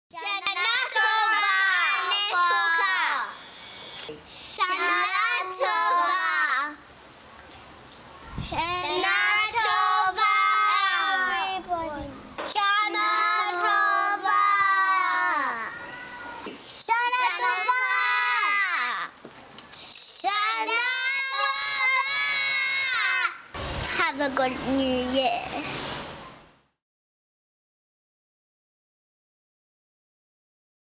Emanuel Woollahra Preschool - Rosh Hashanah Greeting
Emanuel Synagogue

Shana Tova from the children of Emanuel Woollahra Preschool. Join Emanuel Synagogue for High Holy Day services and events online from this Friday